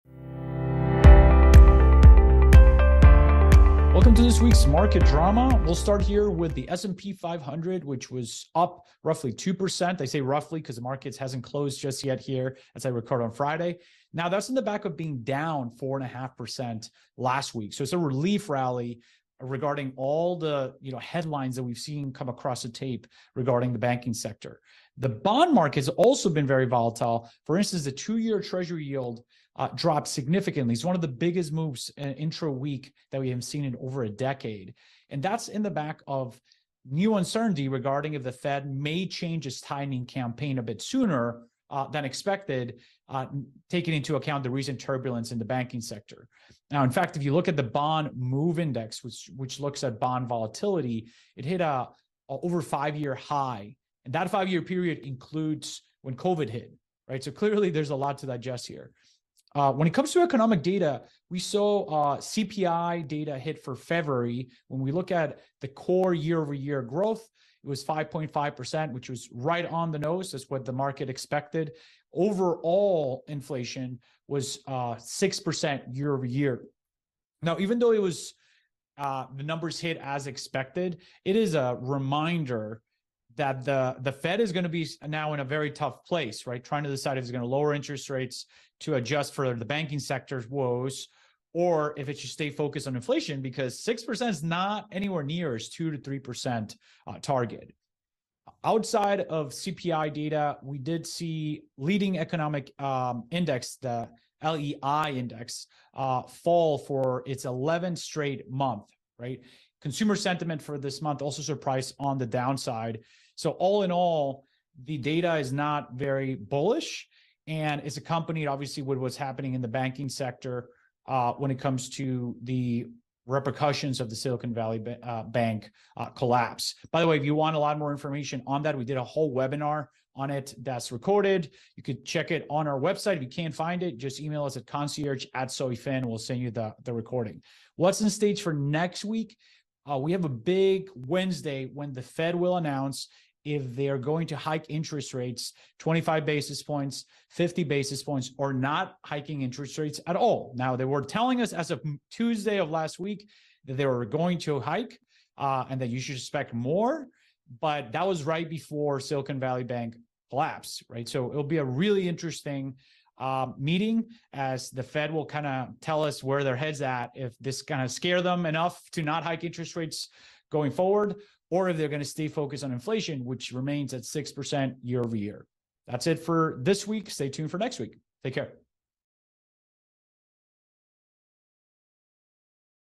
Zoe Financial | Market Drama March 21, 2023
What’s new in the markets this week? Welcome back to Zoe’s weekly Market Drama series, where our CEO & Founder Andres Garcia-Amaya, CFA, provides weekly insights on hot topics and the latest trends in the financial market.

Zoe’s meticulously curated network includes fiduciaries, financial advisors, and financial planners.